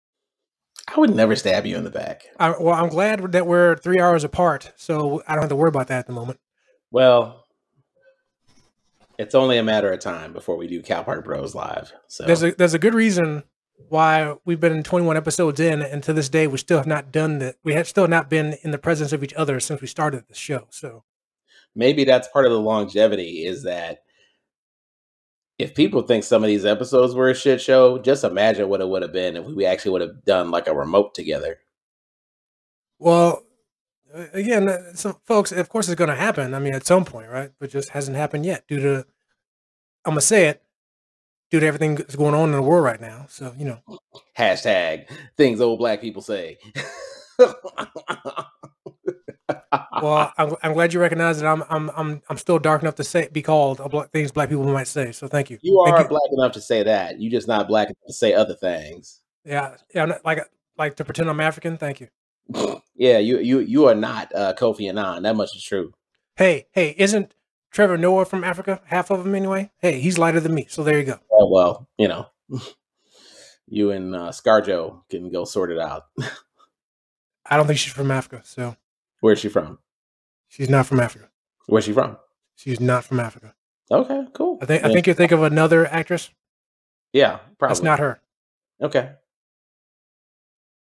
Cal Park Bros Between The Segments: Where She From?
One of the Cal Park Bros didnt realize Scarlett Johansson isnt from Africa.